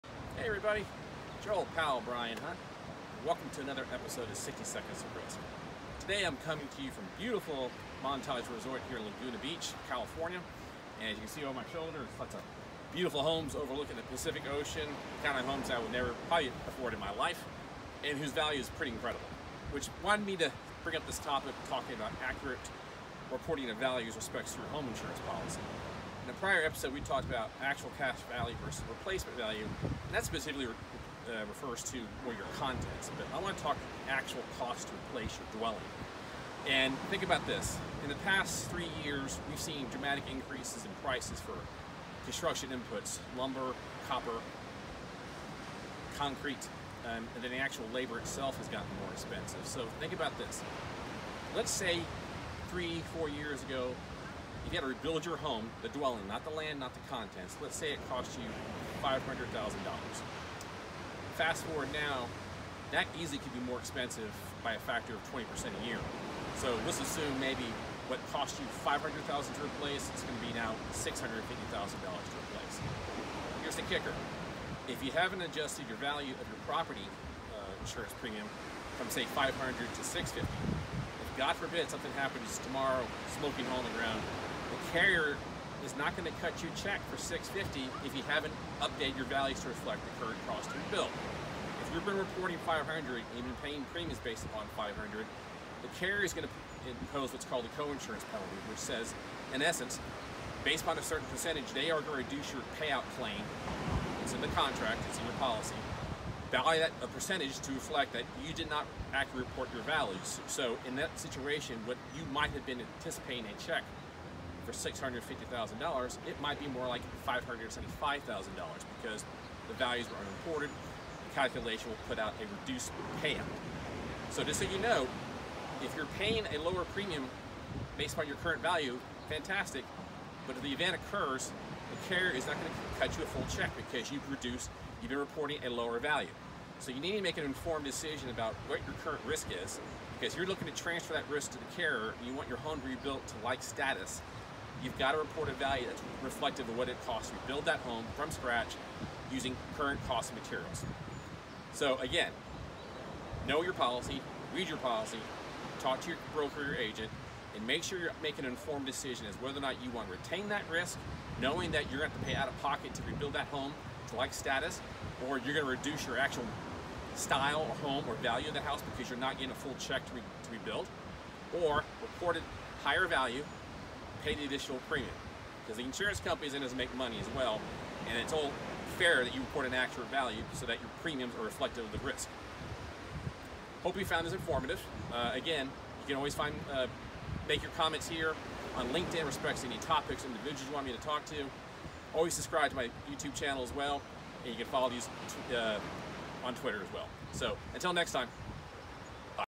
Ep. #15 - Fundamentals of the Coinsurance Penalty
In this episode, we discuss why it's important to make sure you are reporting accurate values with respects to your property - both residential and commercial. Specifically, so you can avoid a coinsurance penalty in the event of a claim.